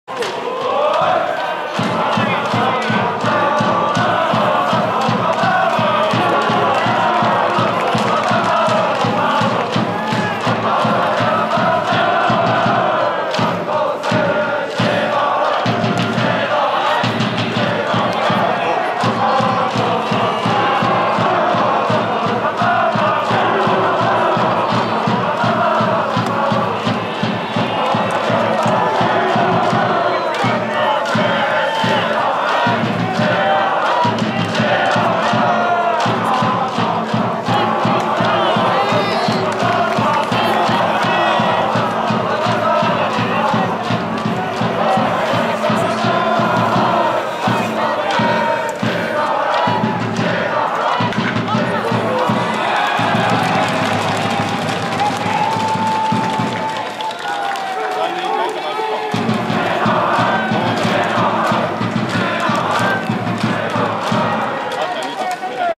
2017/10/30 篠原 涼 （筑波大学） 横浜市長杯・城西国際大学戦での右安打 2年秋
篠原 涼　（福井・敦賀気比高校 ⇒ 筑波大学）　167cm70kg　右左　2年生　1番 サード

横浜市長杯1回戦　四球、右安、一ゴロ、左飛